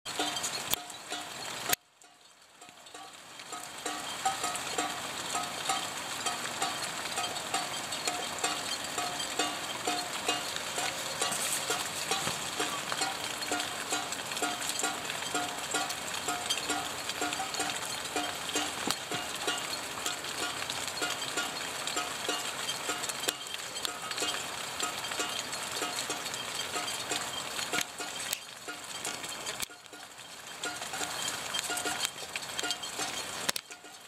Mushroom rain harp
My homemade mushrooms and other metal mushrooms given to me for retirment make up my Rain Harp. It's lined up under the roof of my garage.